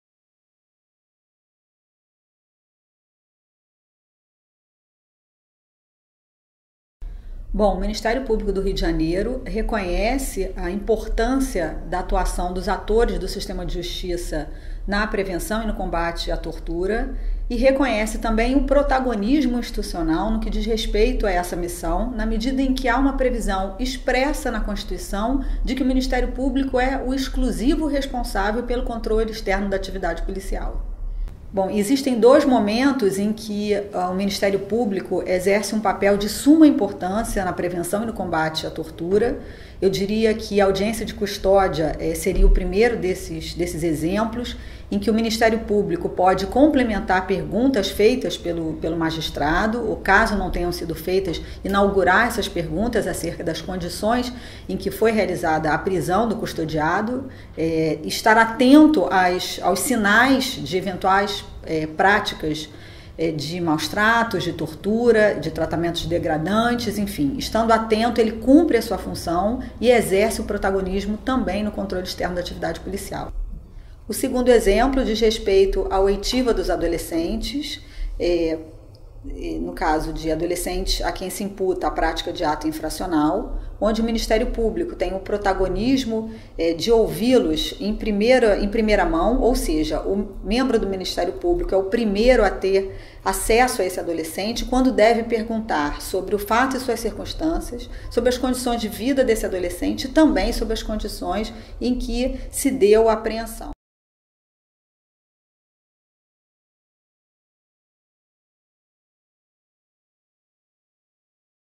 Como é a atuação do MPRJ no combate à tortura?
A promotora de Justiça Eliane de Lima Pereira, coordenadora da Assessoria de Direitos Humanos e Minorias, fala sobre a atuação do MPRJ no combate à tortura.